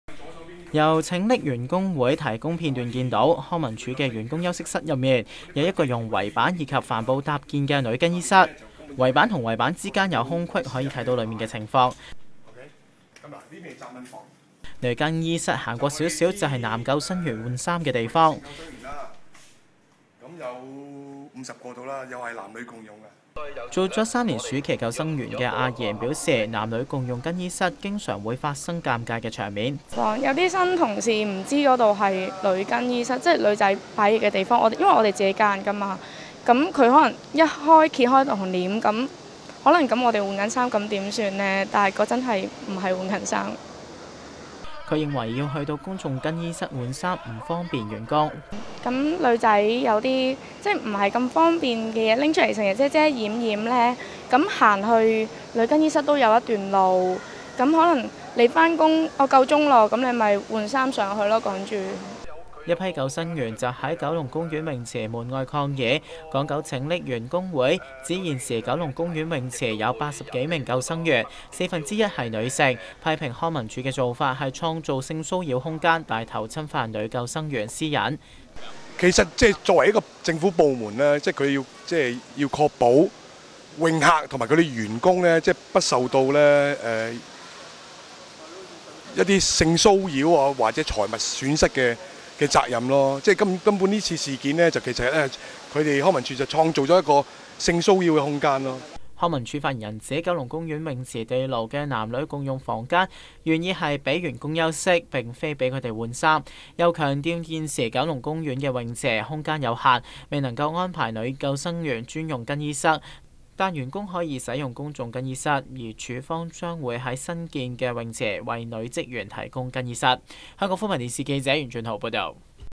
救生員反男女共用更衣室
一批救生員到九龍公園泳池外抗議，不滿康文署安排男女救生員共用更衣室，認為政府帶頭侵犯女救生員私隱，創造性騷擾空間，而康文署否認。